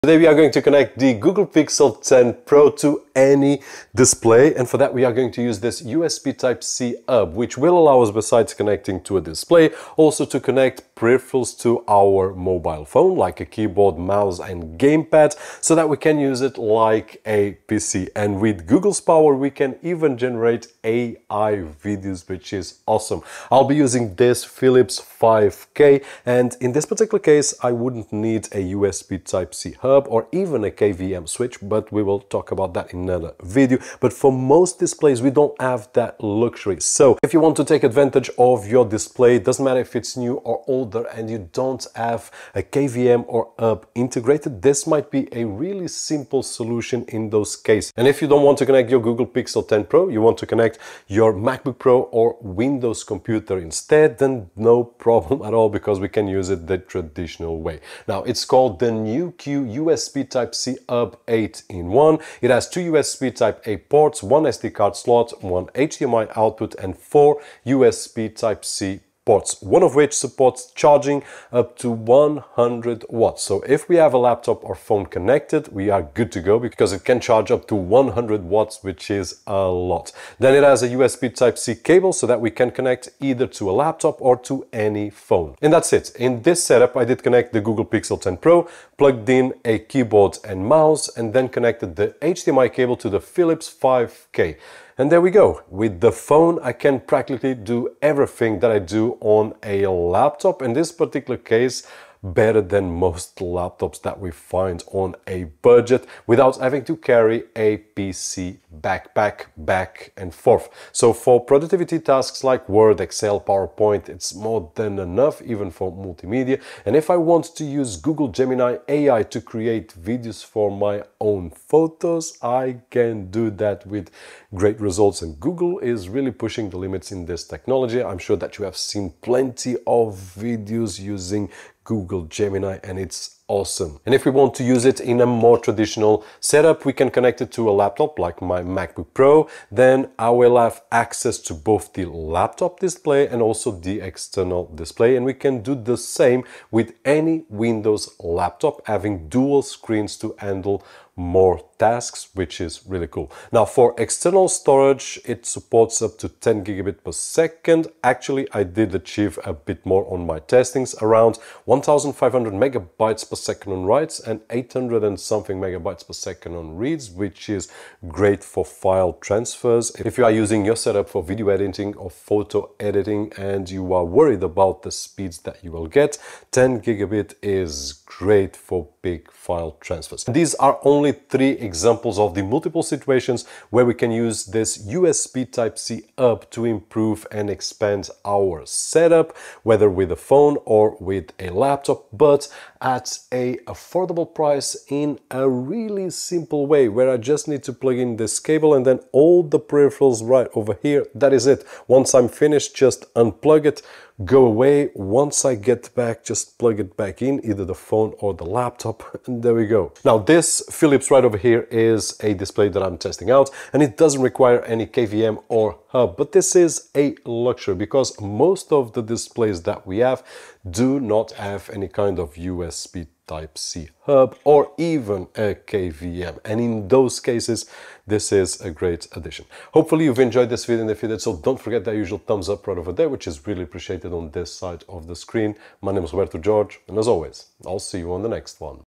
🔌 Turn Your Phone into a PC with This USB C HUB! NEWQ | Google Pixel 10 Pro + 5K Monitor 😱
✅ RECORDING GEAR: ✅